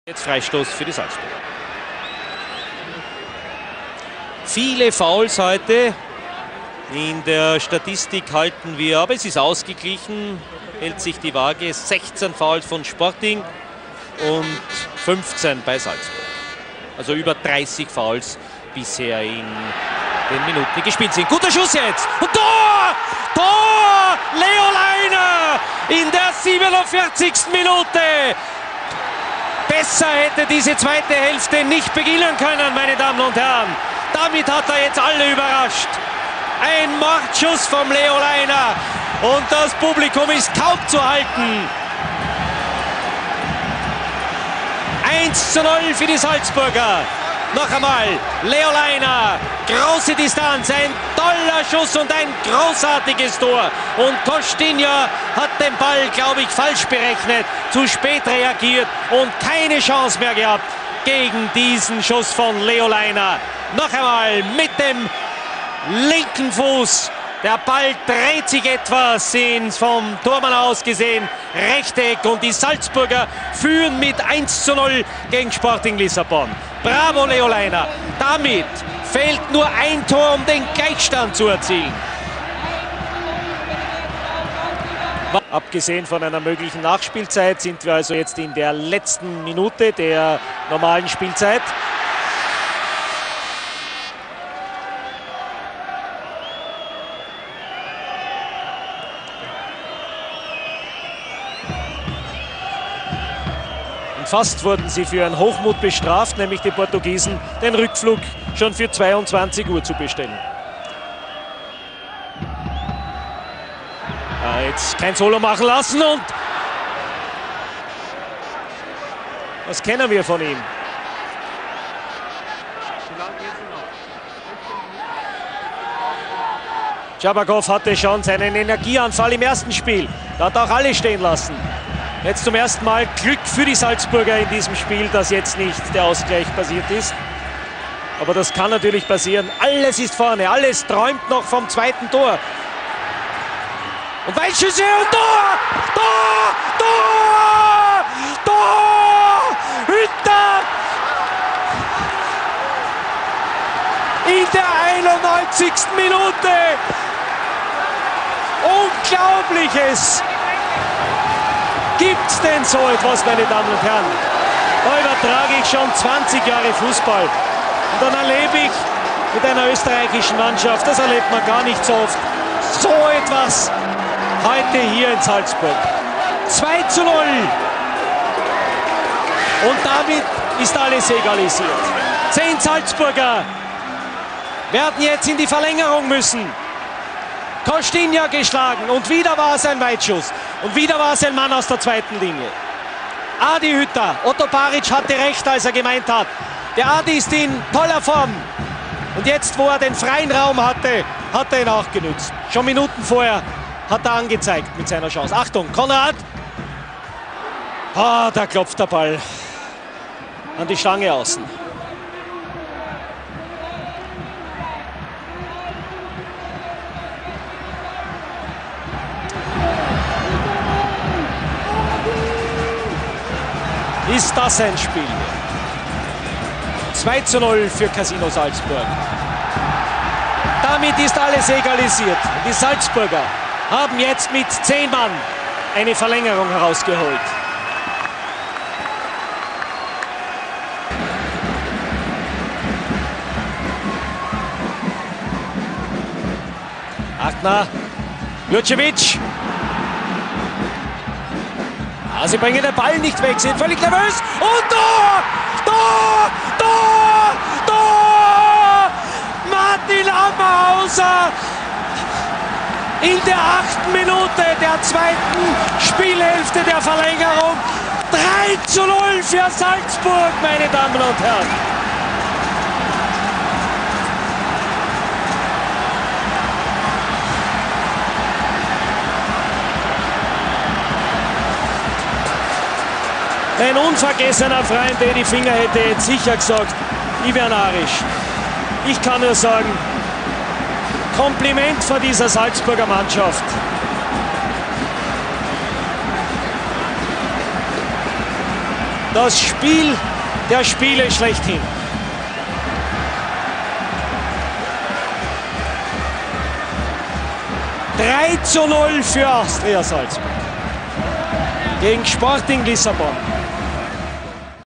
EC 93/94: Austria Salzburg - Sporting Lissabon 3:0
Zusammenfassung der Tore vom UEFA-Cup-Spiel Austria Salzburg - Sporting Lissabon am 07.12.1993; Salzburg siegte in der Verlängerung, Hinspiel: 0:2

compilation of the goals from the 3-0 home win of Austria Salzburg against Sporting Lisbon on December 7th, 1993; after a 0-2 defeat in the first leg, Salzburg advanced to the next round after extra time